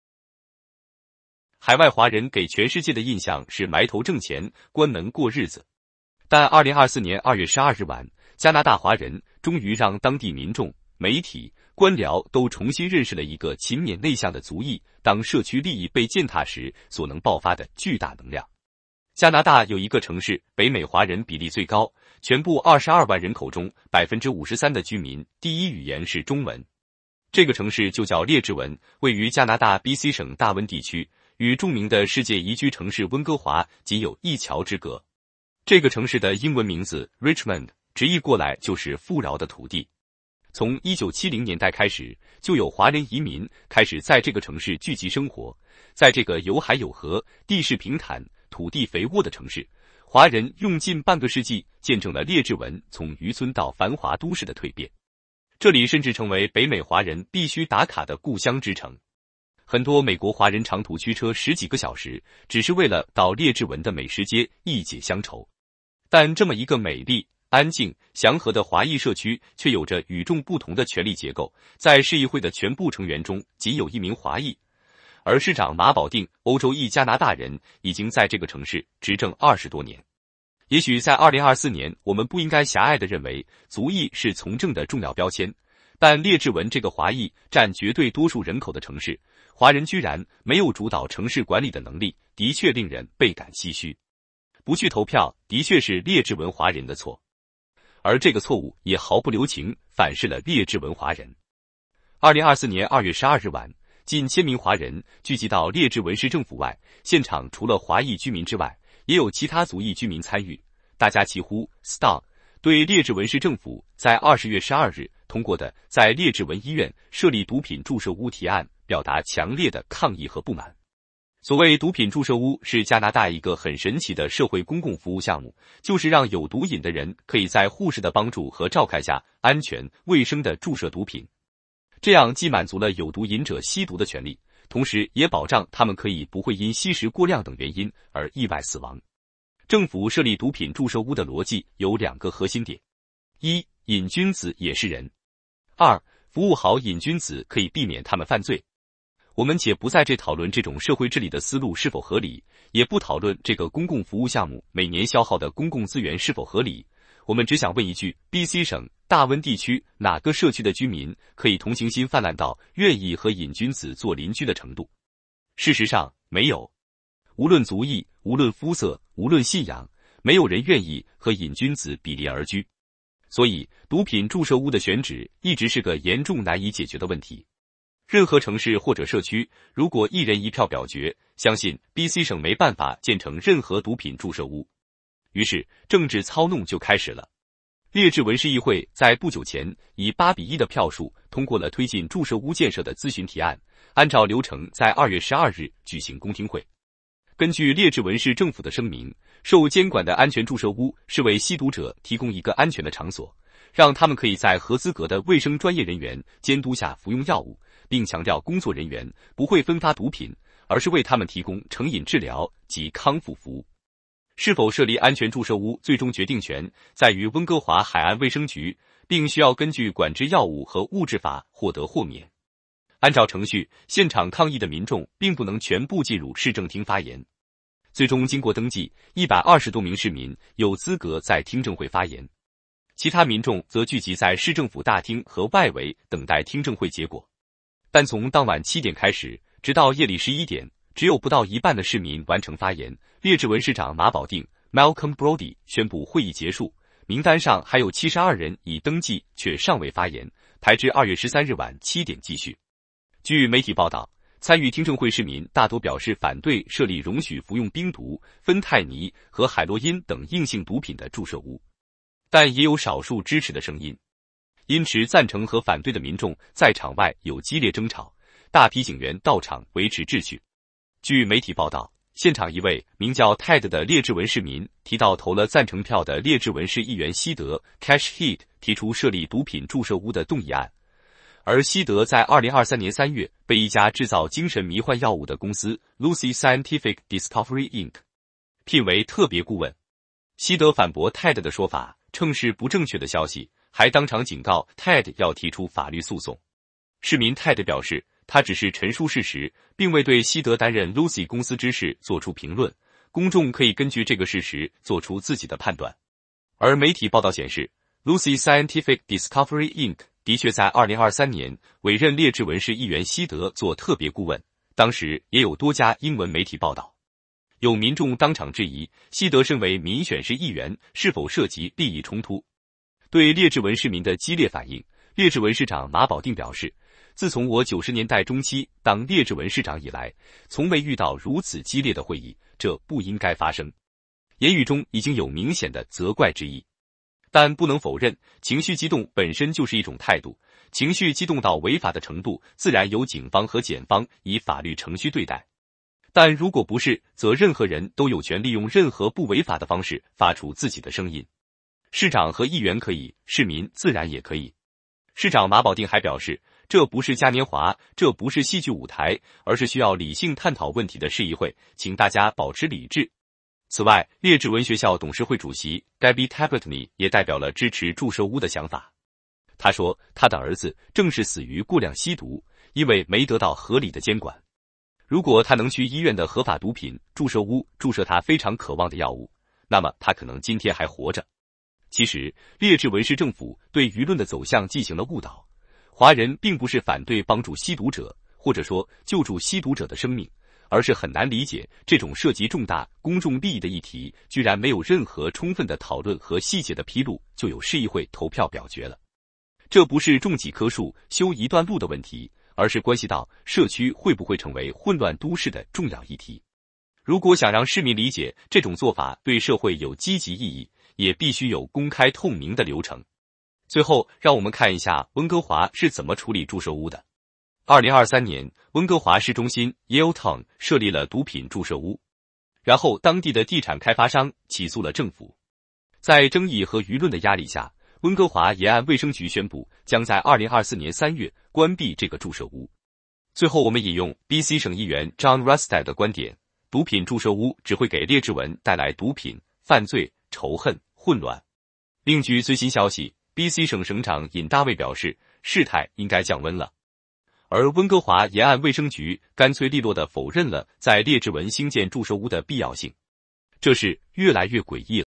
重磅｜加拿大华人 爆发史上最大规模抗议！ 警方出动维持秩序 BC省长呼吁冷静
海外华人给全世界的印象是“埋头挣钱，关门过日子”。但2024年2月12日晚，加拿大华人终于让当地民众、媒体、官僚都重新认识了一个勤勉内向的族裔当社区利益被践踏时所能爆发的巨大能量。